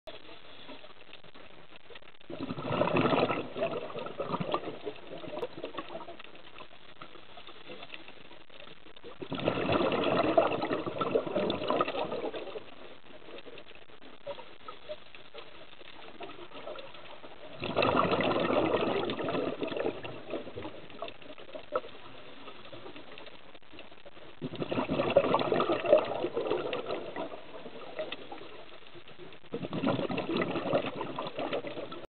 School of old wives
Old wives gossiping on a nice sunny morning :-) Taken 15 April 2014 at Shelly Beach.